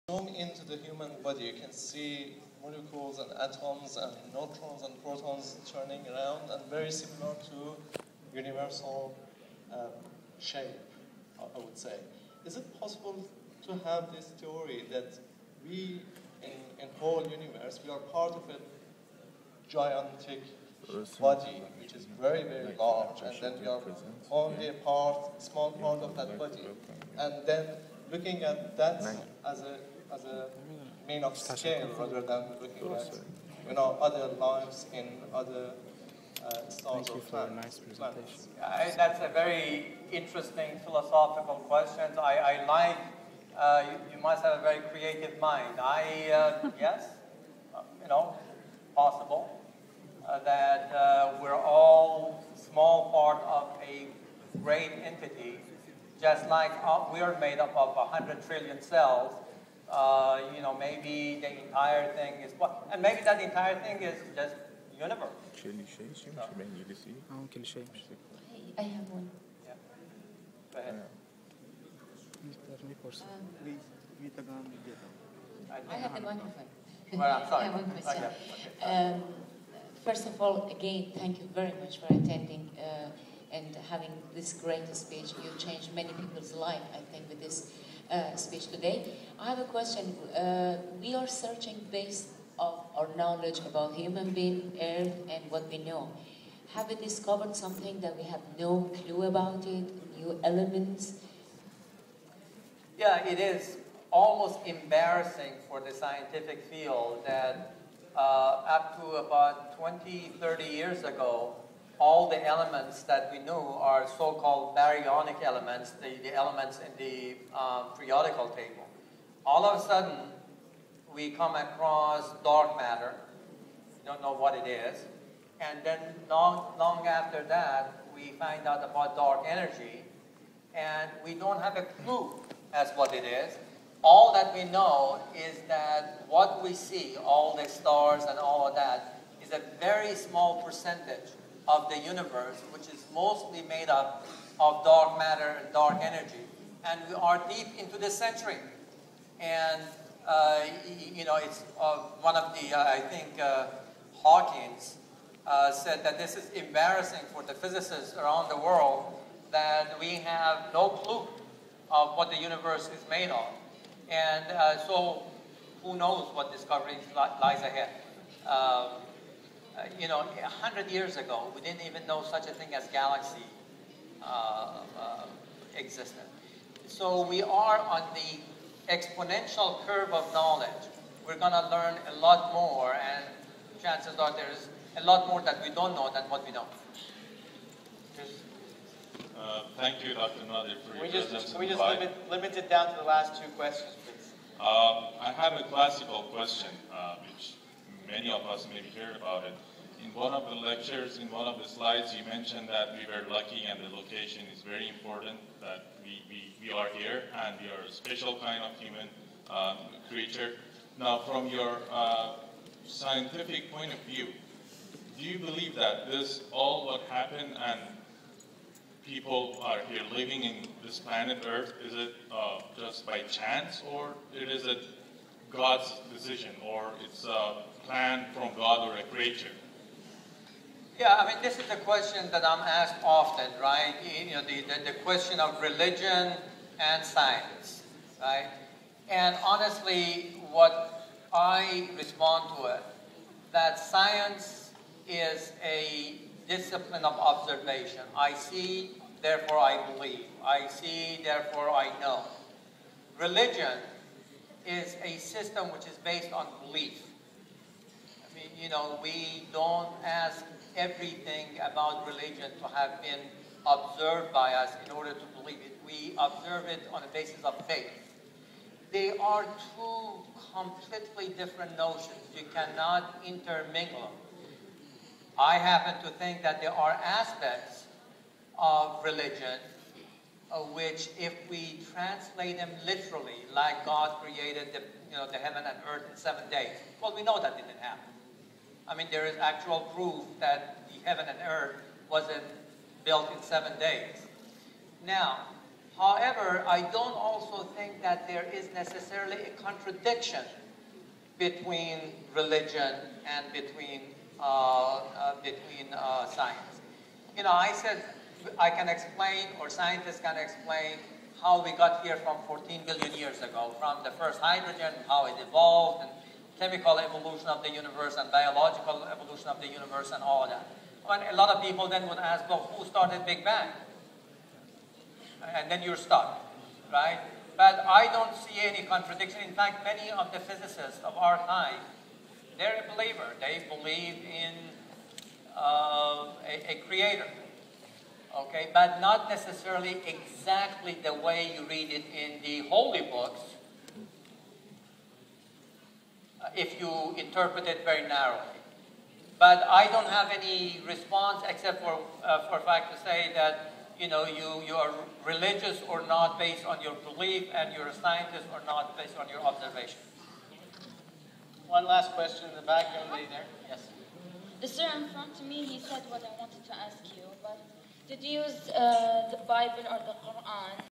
Dr. Naderi Answering My Question: " Where did we come from?"
I Asked Dr. Nadei: " Is it a coincidence or it is planned by superpower (God) that earth is the only living place in the universe"
Dr. Naderi is senior Scientist of NASA and director of JPL in NASA.